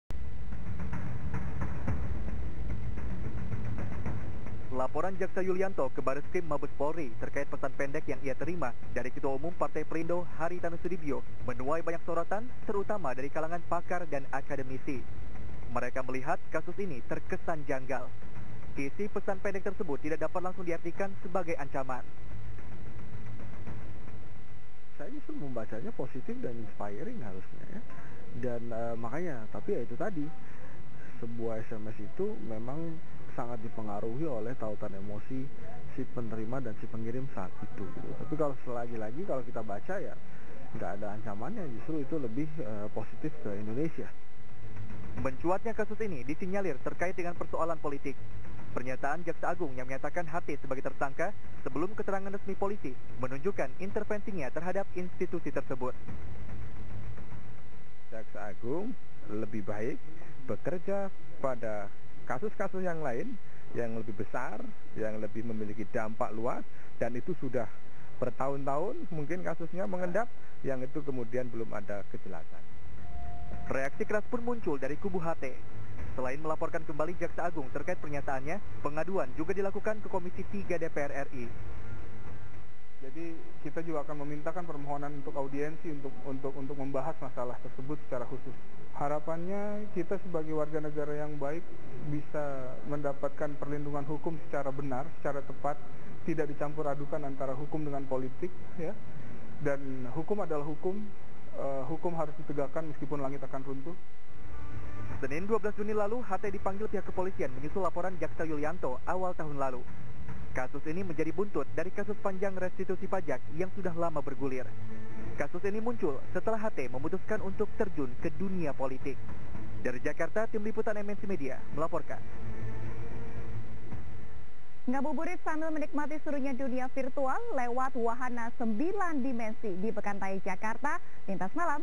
Polemik SMS Ketum Perindo Hary Tanoe Sarat Muatan Politik
Pelaporan Ketum Perindo Hary Tanoesoedibjo terkait SMS yang dikirim kepada Jaksa Yulianto terkesan politis. Hal ini terlihat dari beberapa hal, salah satunya penyebutan Hary Tanoe sebagai tersangka oleh Jaksa Agung yang mendahului penyidikan kepolisian.